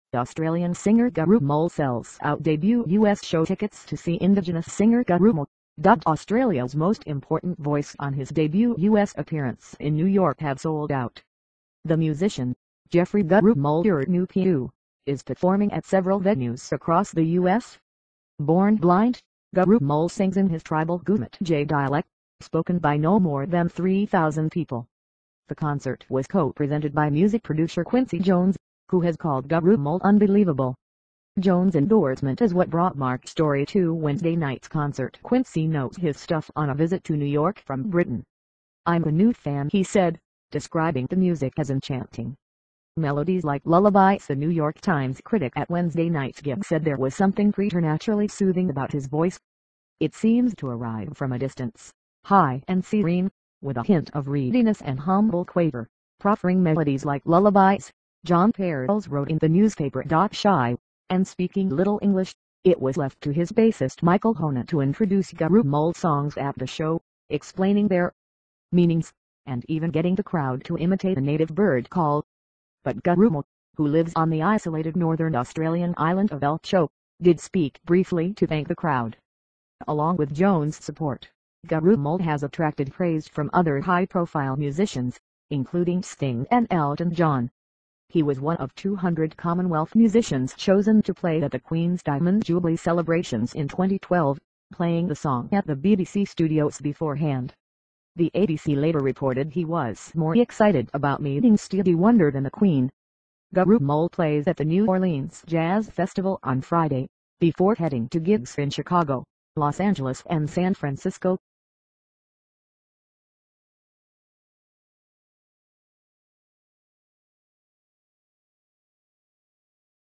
Australian singer Gurrumul sells out debut US show
Tickets to see Indigenous singer Gurrumul - dubbed "Australia's most important voice" - on his debut US appearance in New York have sold out.
The musician, Geoffrey Gurrumul Yunupingu, is performing at several venues across the US.
Born blind, Gurrumul sings in his tribal Gumatj dialect, spoken by no more than 3,000 people.
The concert was co-presented by music producer Quincy Jones, who has called Gurrumul "unbelievable".
Jones' endorsement is what brought Marc Storey to Wednesday night's concert ("Quincy knows his stuff"), on a visit to New York from Britain.
"I'm a new fan", he said, describing the music as "enchanting".
'Melodies like lullabies'
A New York Times critic at Wednesday night's gig said there was something "preternaturally soothing" about his voice.
"It seems to arrive from a distance, high and serene, with a hint of reediness and a humble quaver, proffering melodies like lullabies," Jon Pareles wrote in the newspaper.Shy, and speaking little English, it was left to his bassist Michael Hohnen to introduce Gurrumul's songs at the show, explaining their meanings, and even getting the crowd to imitate a native bird call.
But Gurrumul, who lives on the isolated northern Australian island of Elcho, did speak briefly to thank the crowd.
Along with Jones' support, Gurrumul has attracted praise from other high-profile musicians, including Sting and Elton John.
He was one of 200 Commonwealth musicians chosen to play at the Queen's Diamond Jubilee celebrations in 2012, playing a song at the BBC studios before hand.
The ABC later reported he was more excited about meeting Stevie Wonder than the Queen.
Gurrumul plays at the New Orleans Jazz Festival on Friday, before heading to gigs in Chicago, Los Angeles and San Francisco.